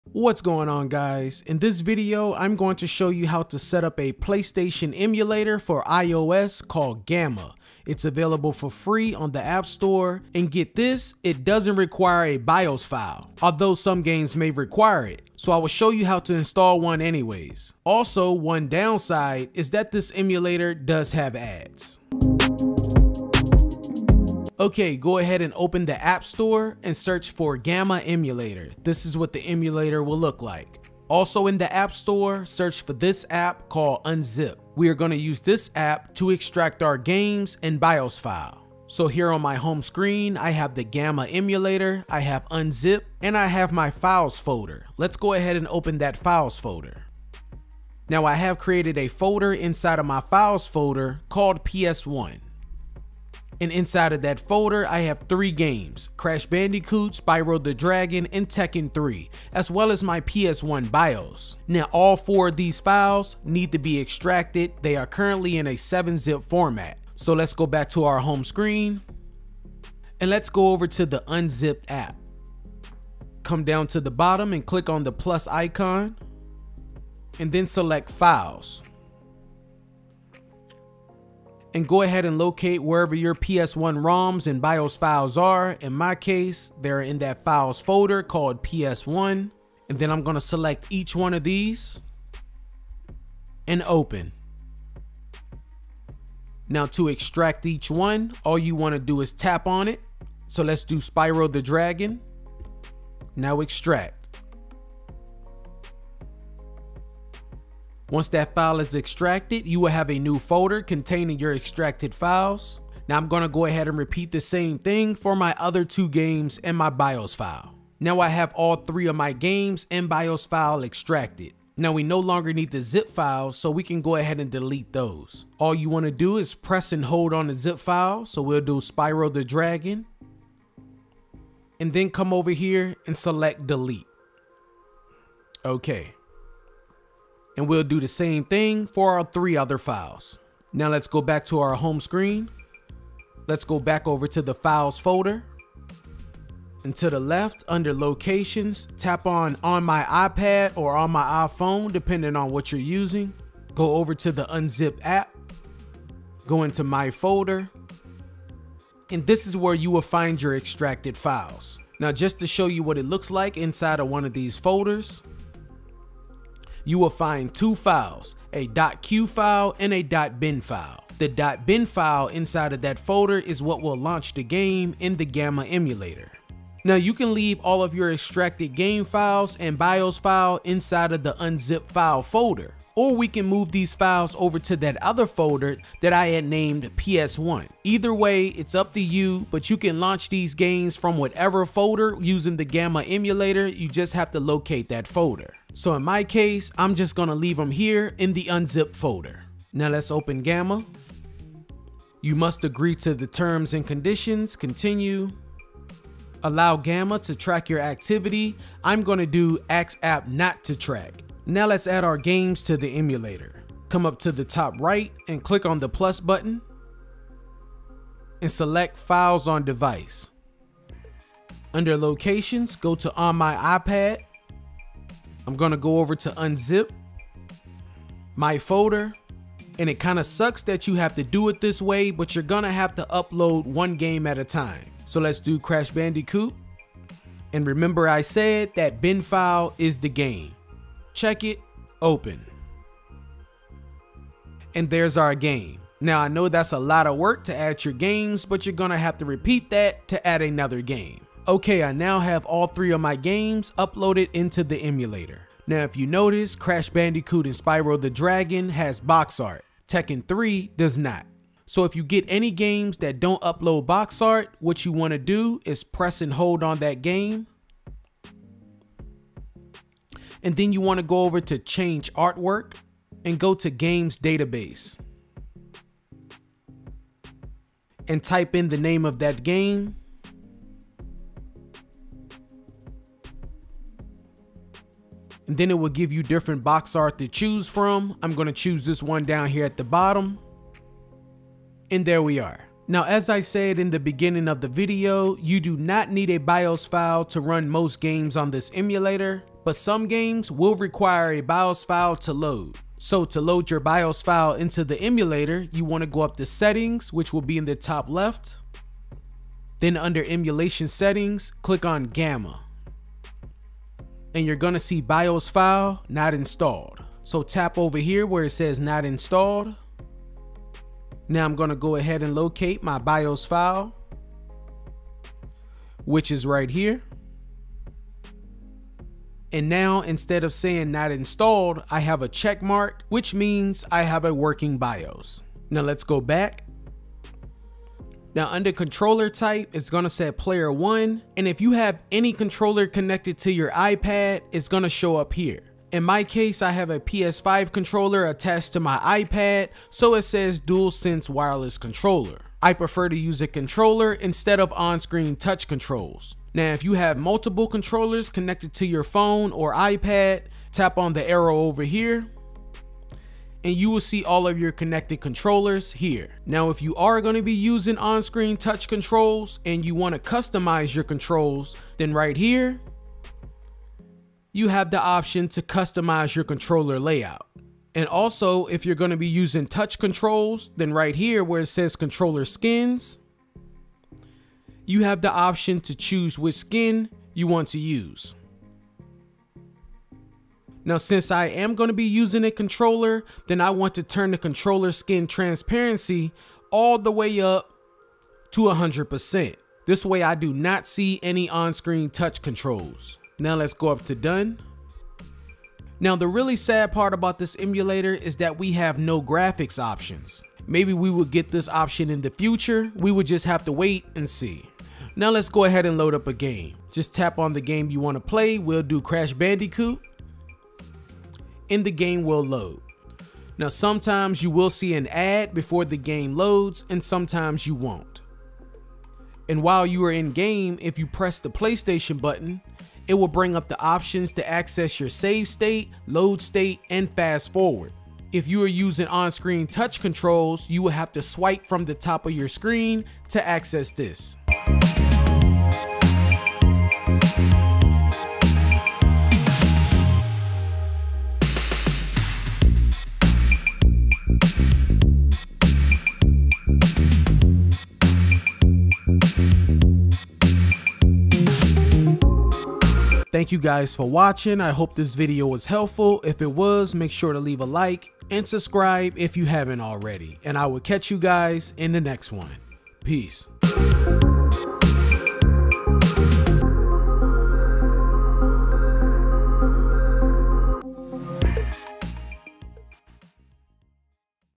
PS1 Emulator Gamma Setup For IOS on iPad Or iphone
You can Download Gamma and Unzip from the App Store